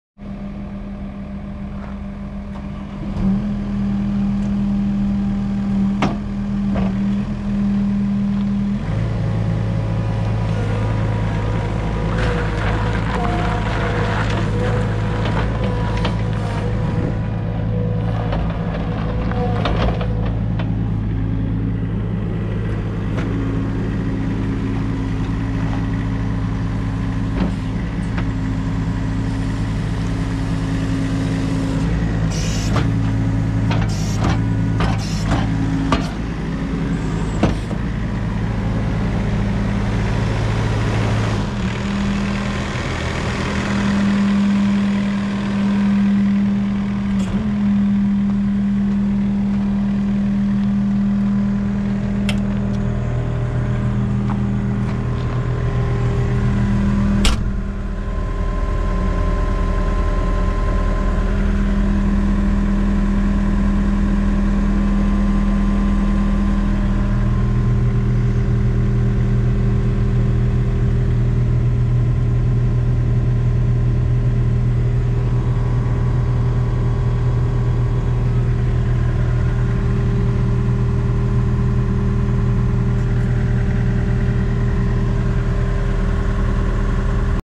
1091 - 2017 Deere 318G Skid Steer Will Be Sold At Auction!
2017 Deere 318G Skid Steer Will Be Sold At Auction! for all the auction details!